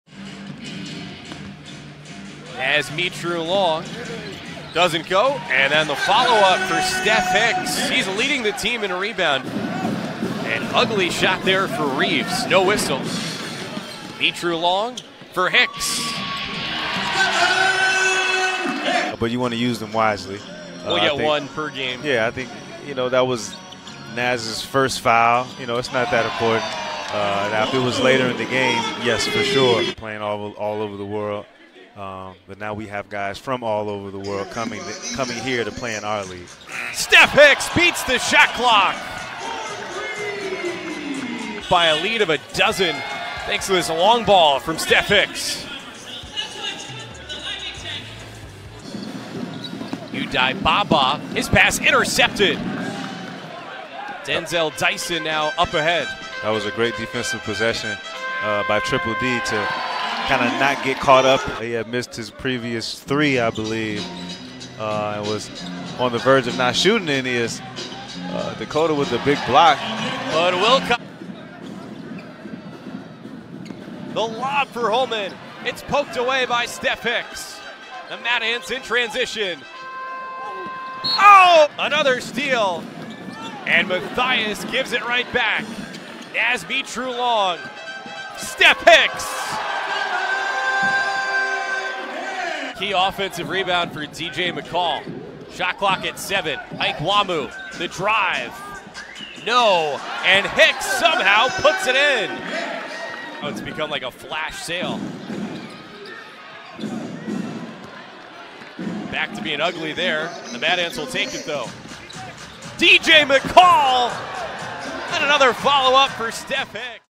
Stephan Hicks Posts 18 points and 14 rebounds vs Texas Legends - 11/29/2019
About the Fort Wayne Mad Ants